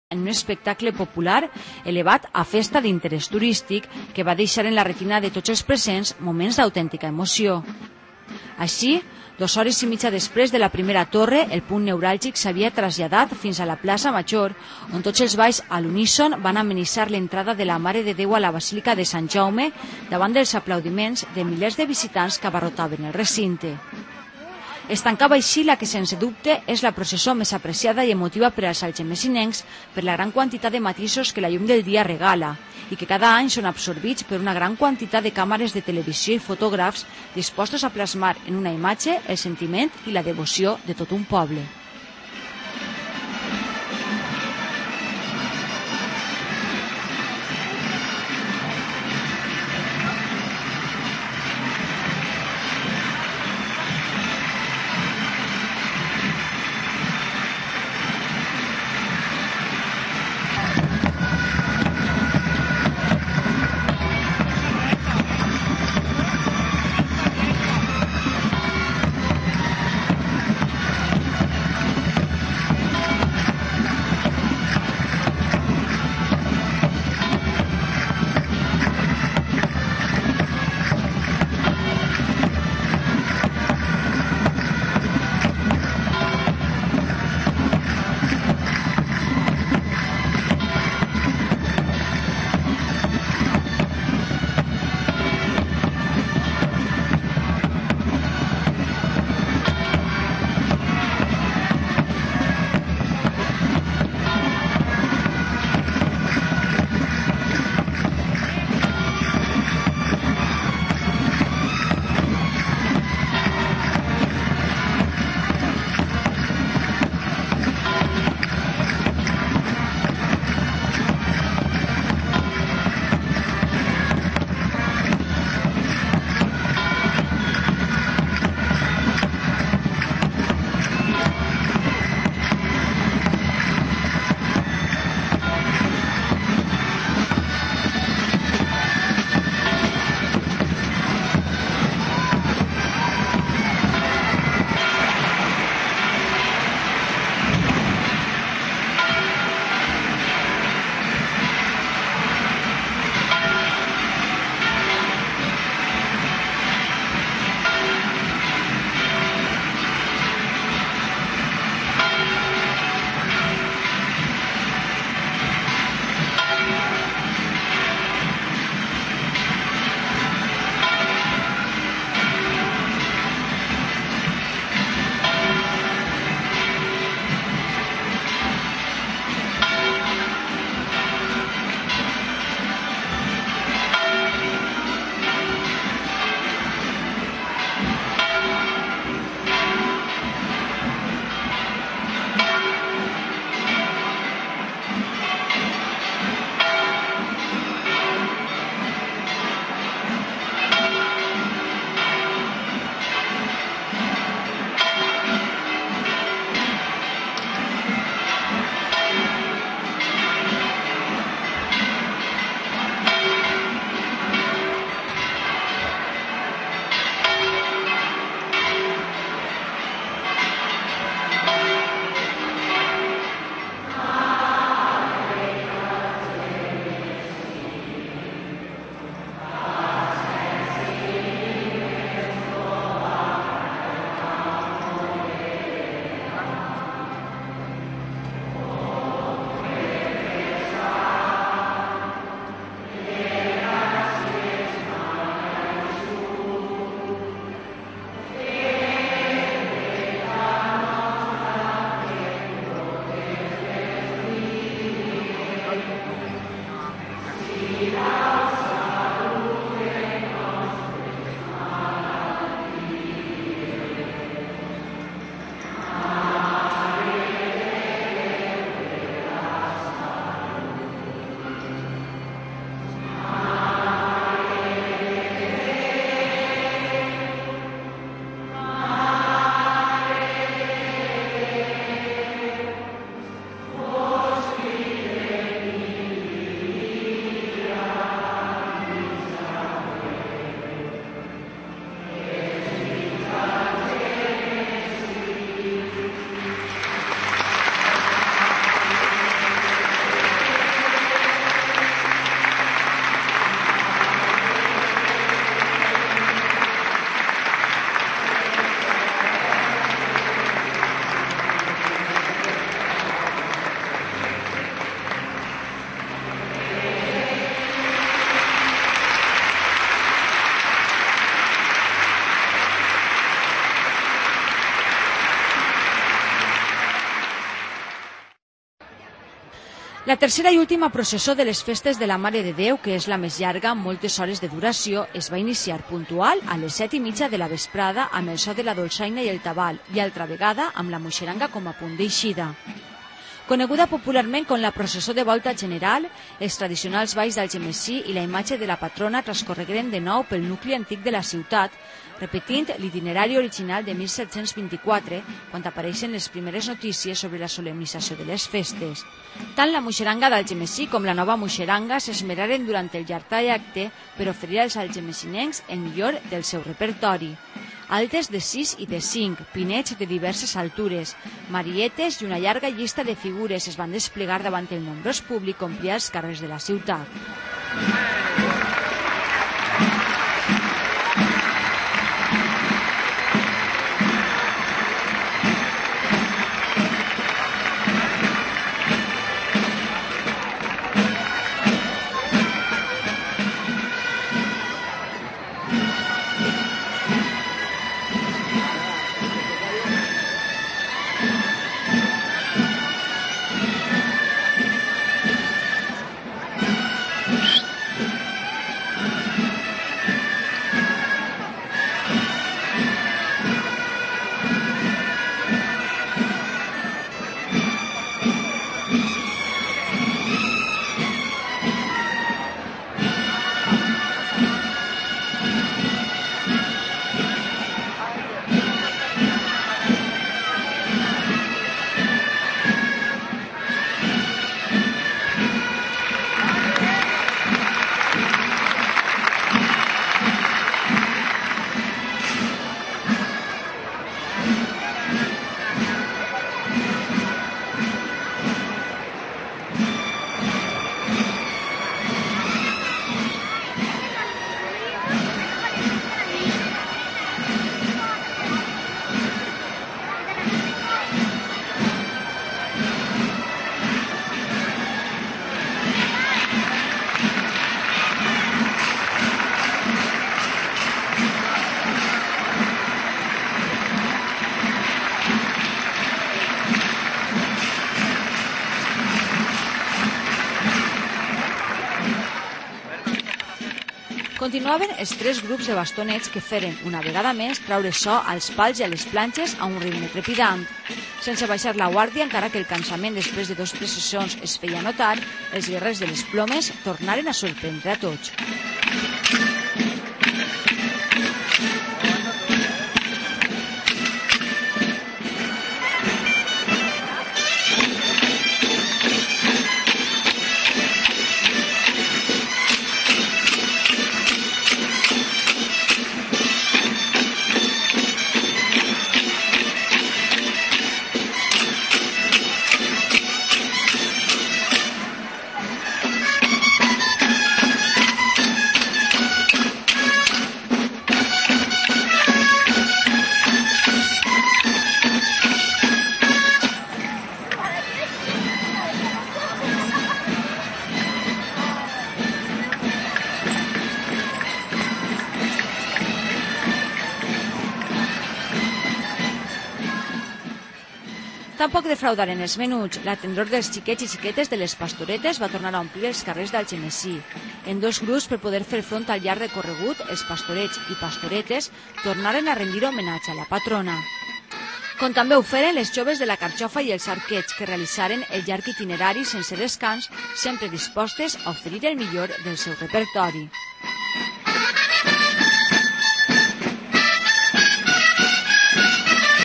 Mare de Déu 2008 - Berca TV (4/6)
Reportatge sobre les Festes en Honor a la Mare de Déu de la Salut d'Algemesí emès per Berca Televisió.